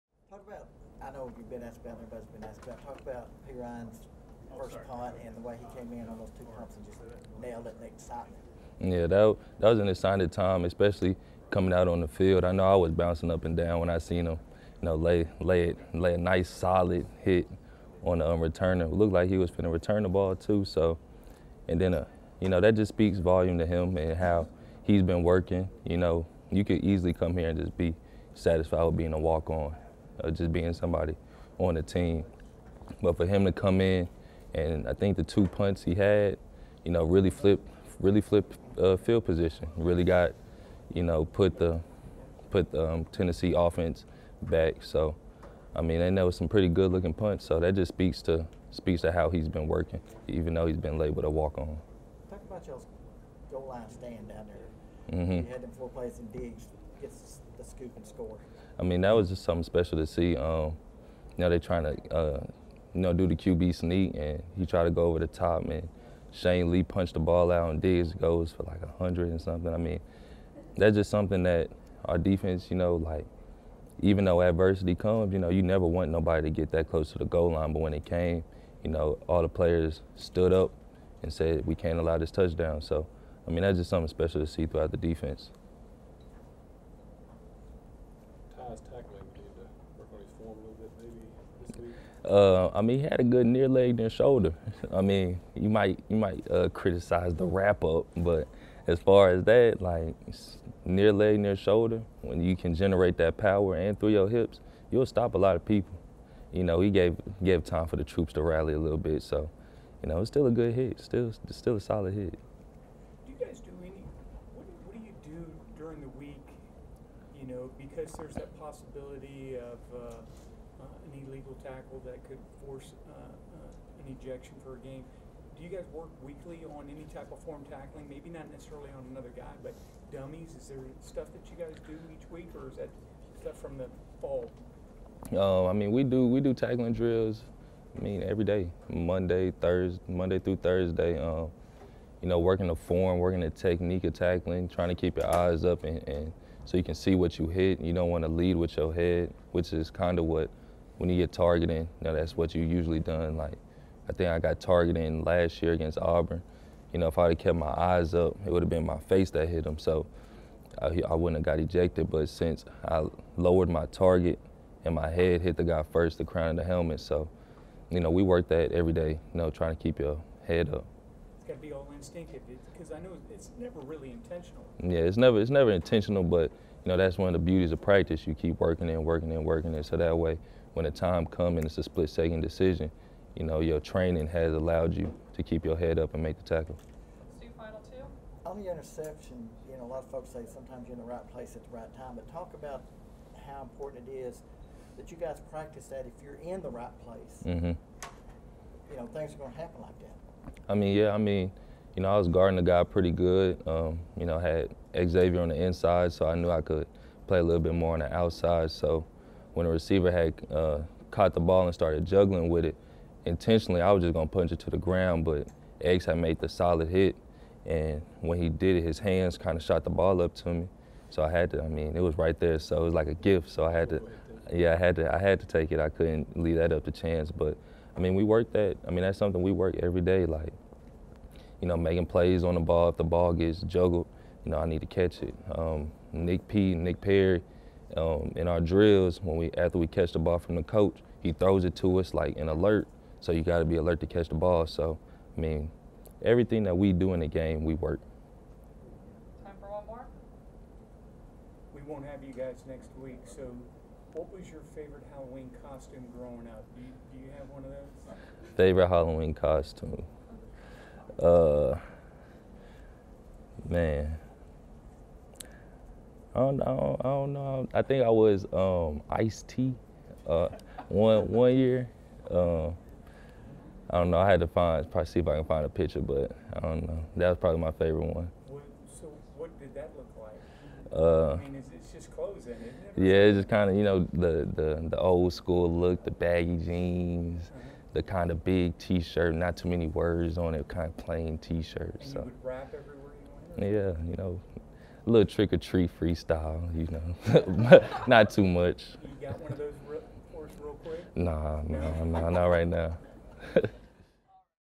Jared Mayden addresses the media as Alabama prepares for Arkansas
Alabama DB Jared Mayden addresses the media Monday, as the Crimson Tide prepares for Arkansas.